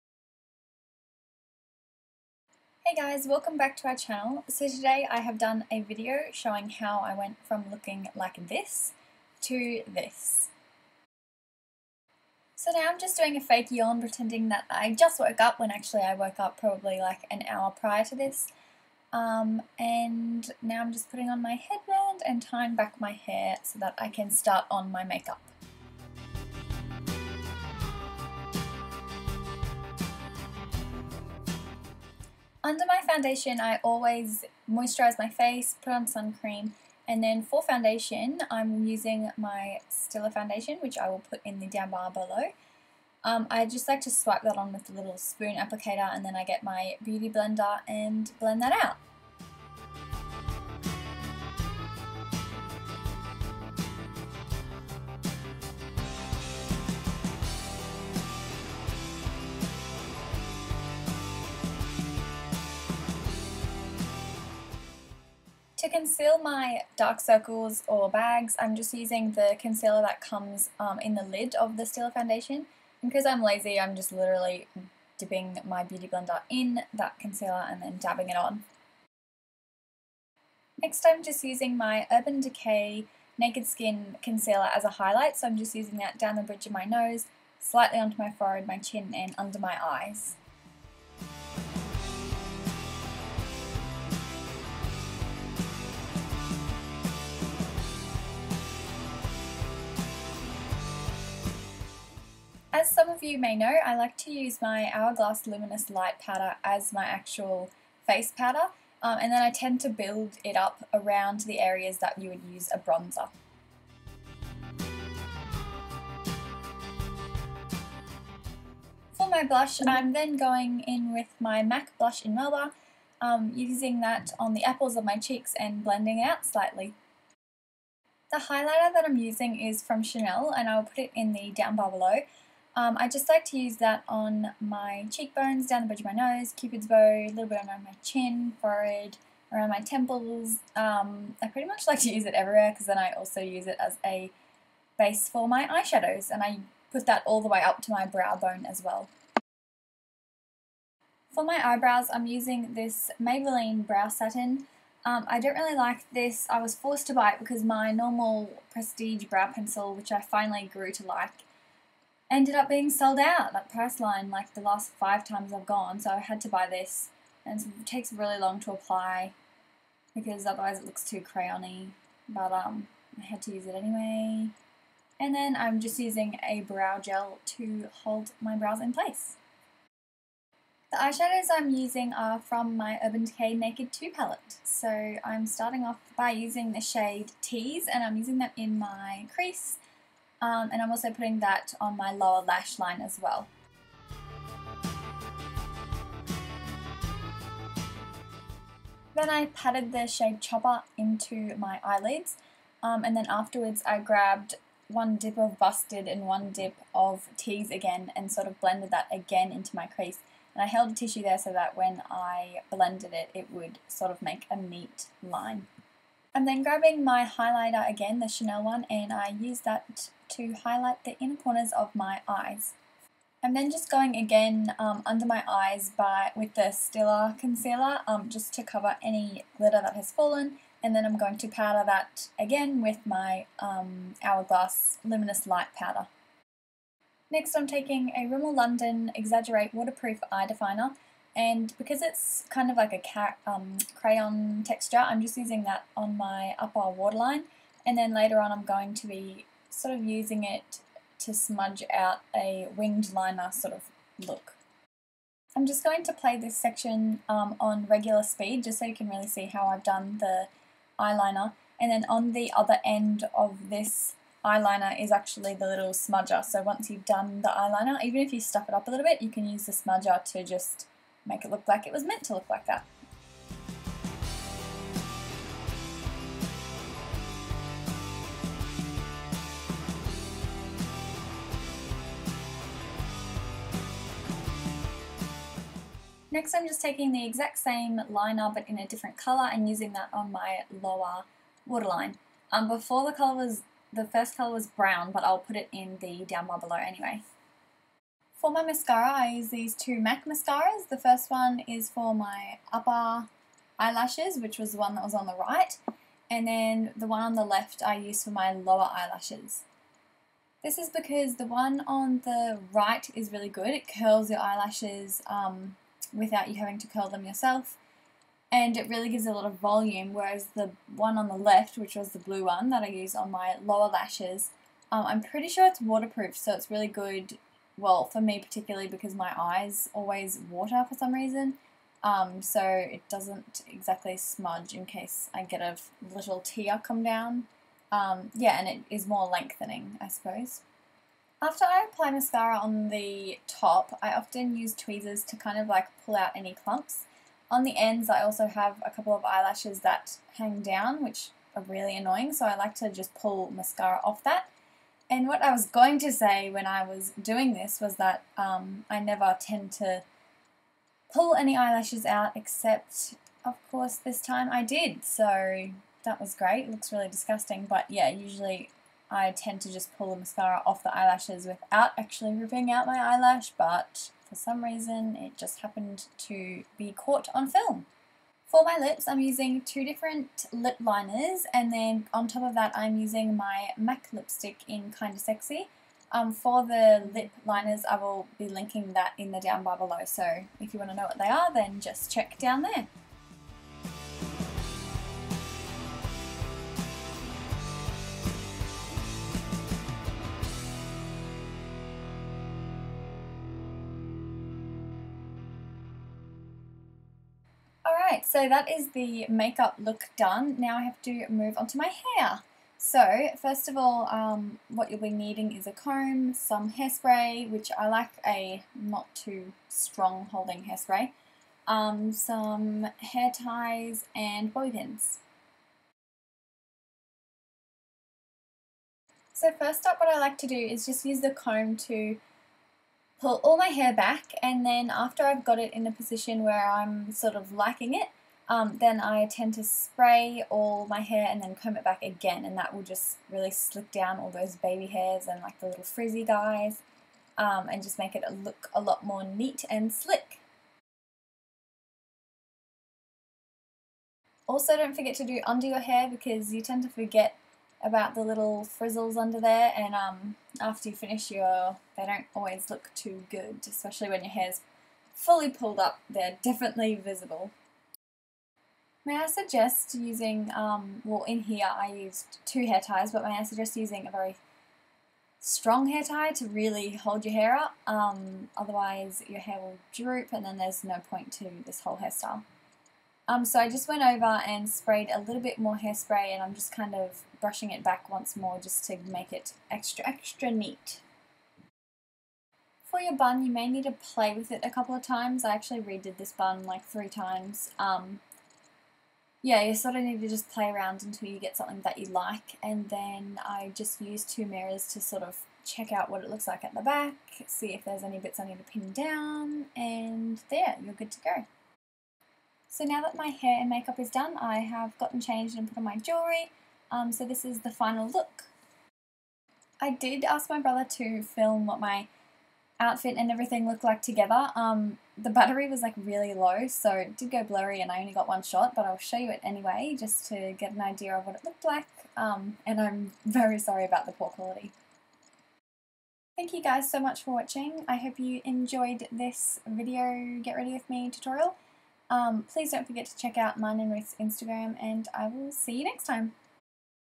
GRWM: From Beast to Beauty
Hey guys! Here is a GRWM makeup tutorial for a wedding I went to recently!

Products used:
Stila Stay All Day Foundation and Concealer
Urban Decay Naken Skin Concealer
Hourglass Luminous Light Powder
Chanel Illuminating Powder "Poudre Signee De Chanel" 5503
MAC blush in Melba
Maybelline New York Brow Satin in Medium Brown
Models Prefer Brow Fixer
Rimmel London Exaggerate Waterproof Eye Definer in 212 Rich Brown
MAC Mascaras in Upward Lash and Extended Play Lash
Lip Liners: Face of Australia in Sugar Rose and Rimmel London 049 Natural

Outfit: Peppermayo.com
Jewellery: Mimco
Shoes: Wittner 'Dion Heels'